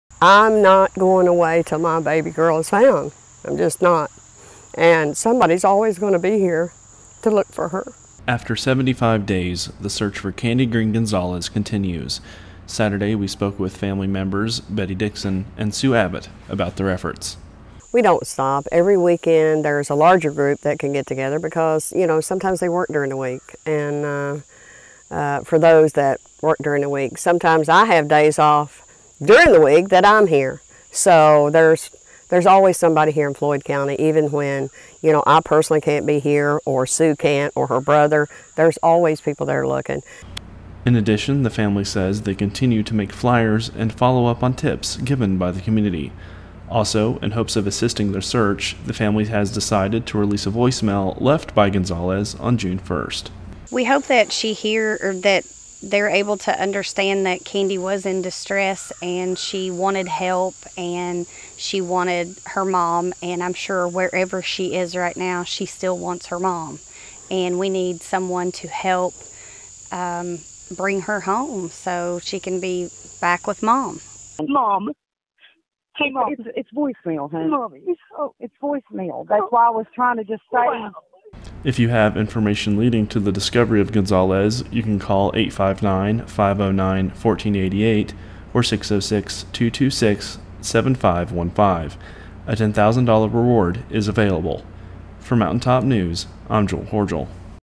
Family of Missing Woman Releases Voicemail to Assist Search
After seventy-five days, the search for Kandi Green Gonzalez continues.